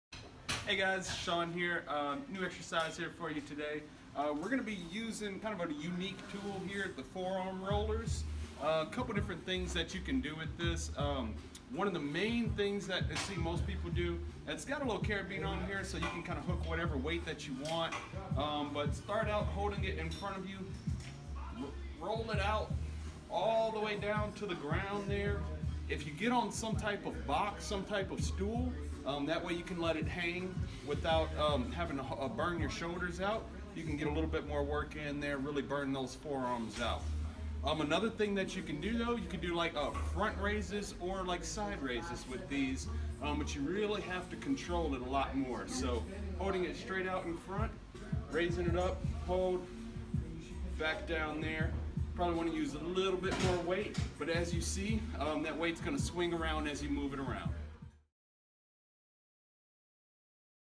Forearm Roller Tips
Shawn shows us some outside the box ways to use the forearm rollers to work more than just the forearms! This is similar to a video we posted previously using resistance bands and kettle bells to do curls. It creates a need for extremely controlled movements to avoid a swing in the weight and to maintain tension!